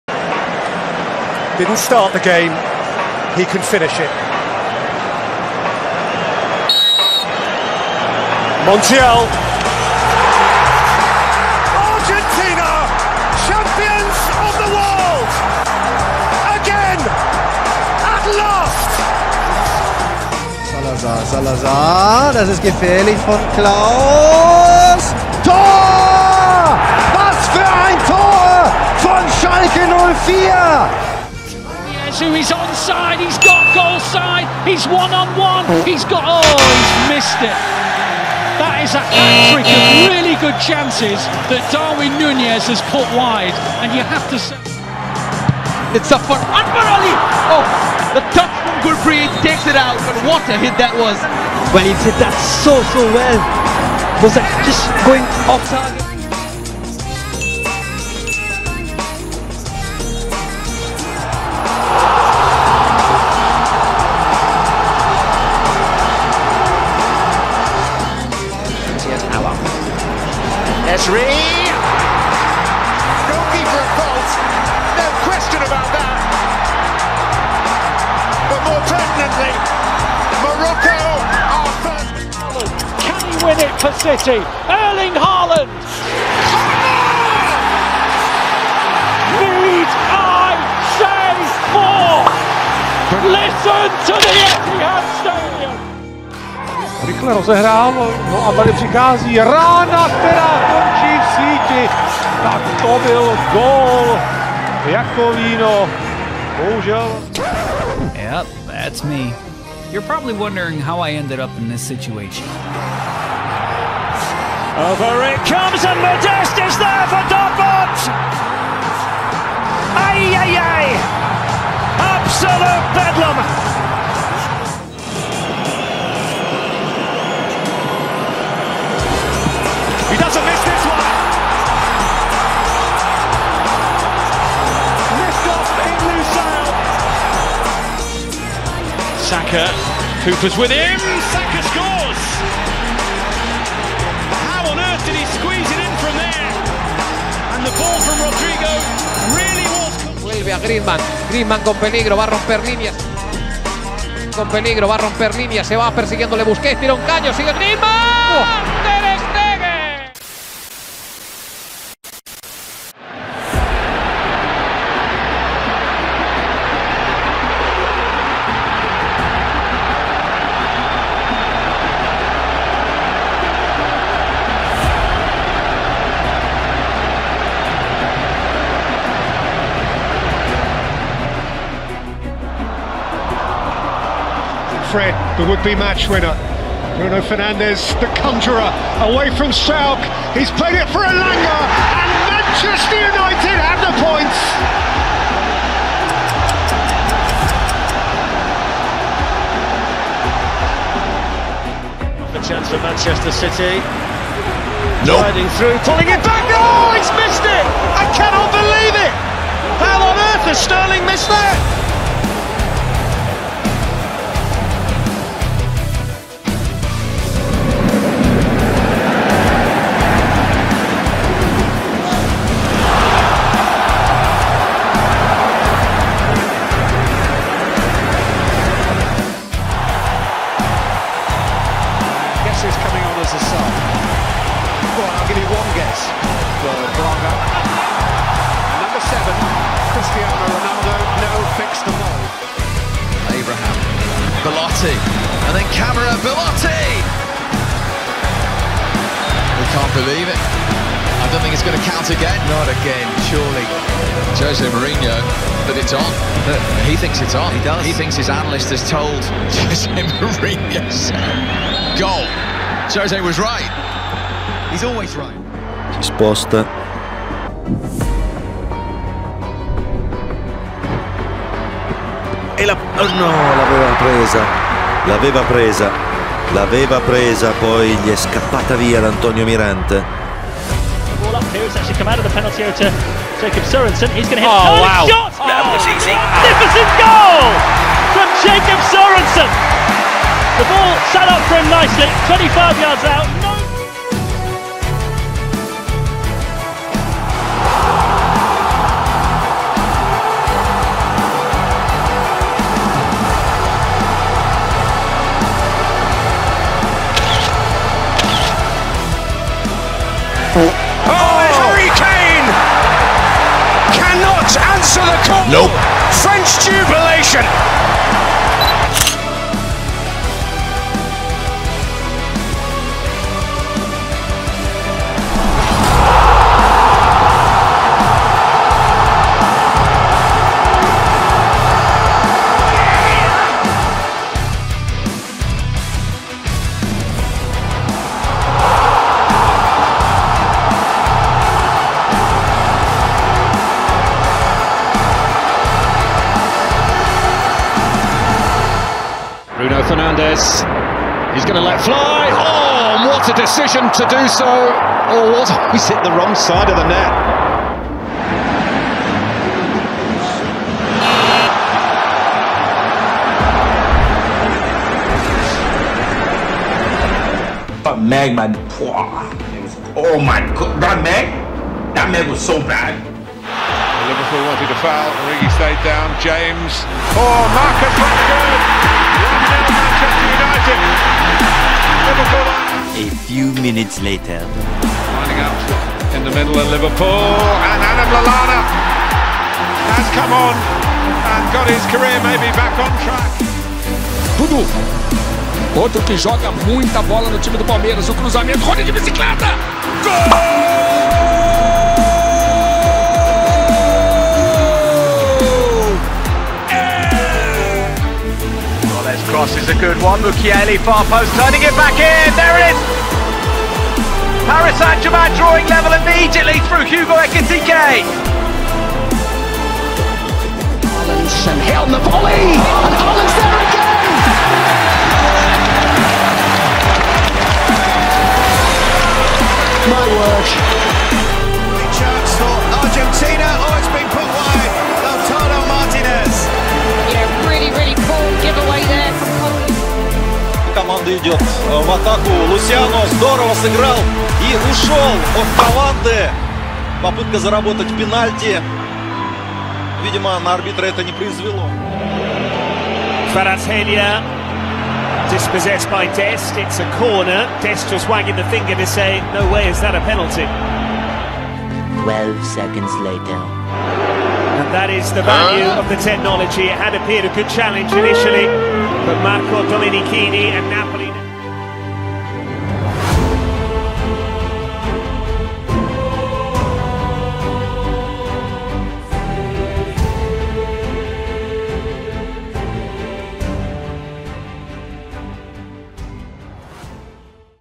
Hyped Reactions In Football
The most Hyped Reactions you've probably seen in Football. All from Ronaldinho, Messi, Neymar any many more!

Produced by @Foot4k